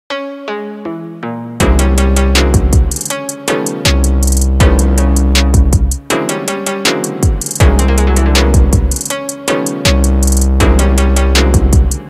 What ATLANTA sounds like on Piano
This is what ATLANTA sounds like on a piano roll in Logic Pro.

What's next?
Write in Comments!